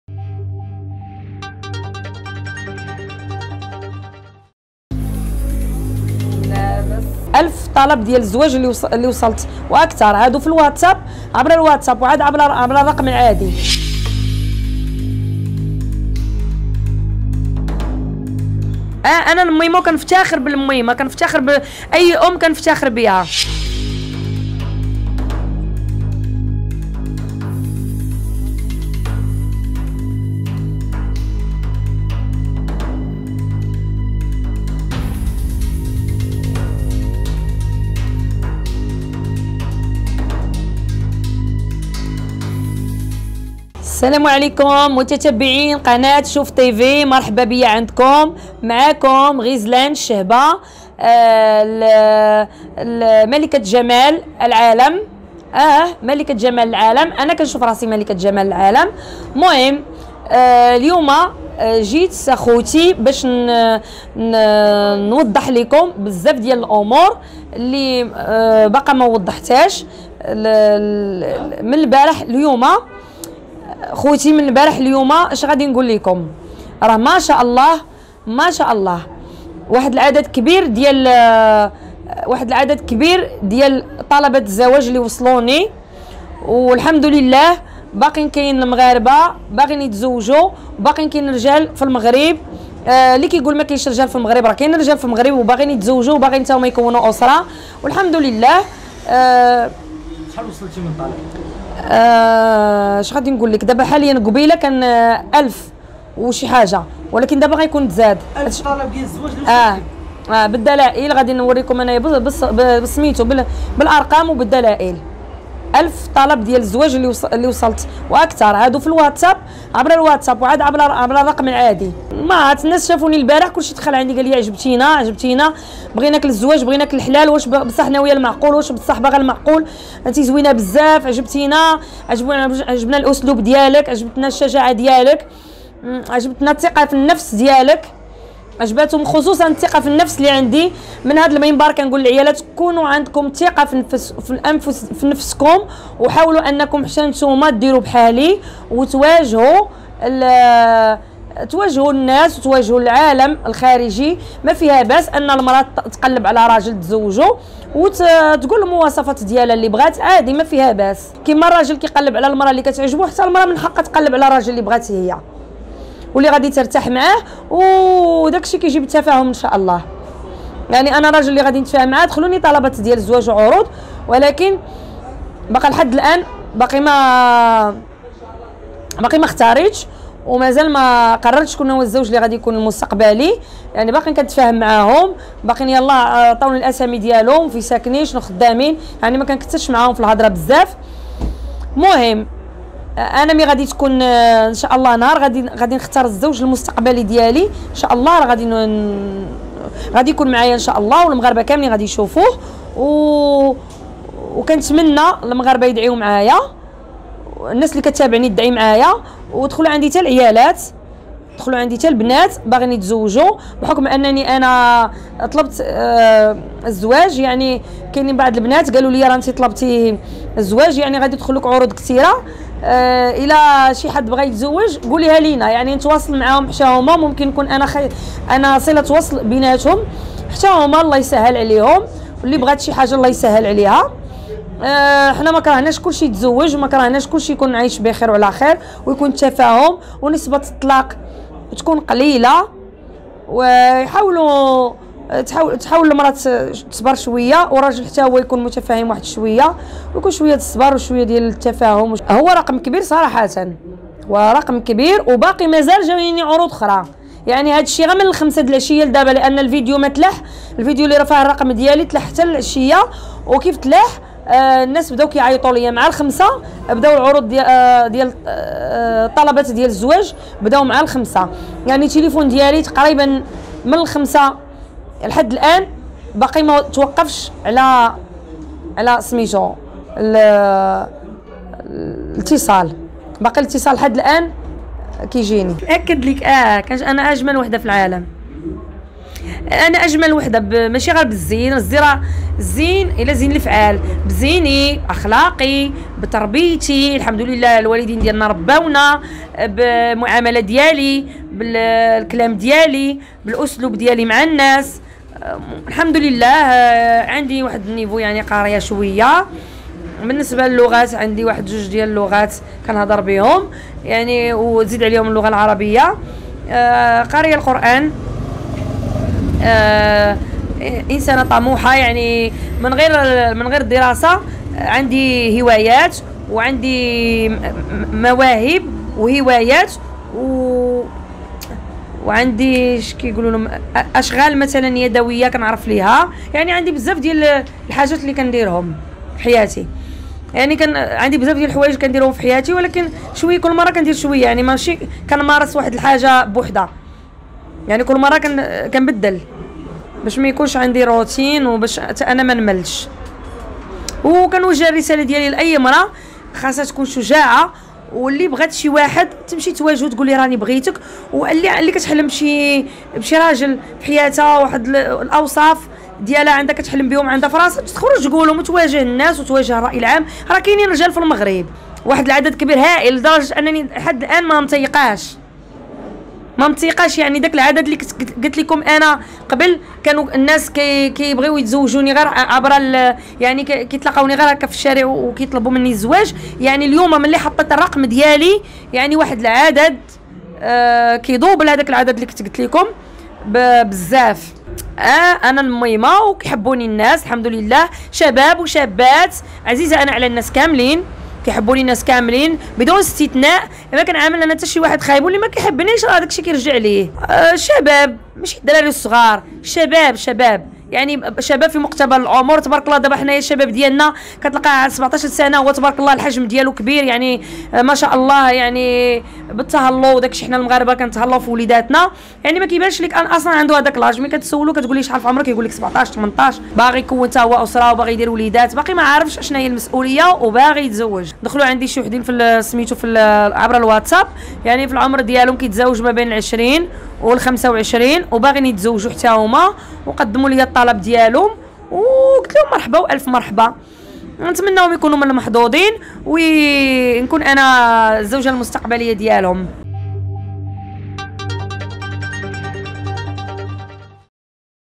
بعد فيديو شوف تيفي.."غزلان الشهبة" دارت الزيف ولقات رجل الأحلام ديالها:"وصلني ألف طلب زواج"
كشفت "غزلان الشهبة" عن تفاصيل خاصة بتوصلها بـألف طلب زواج بعد ظهورها على قناة شوف تيفي.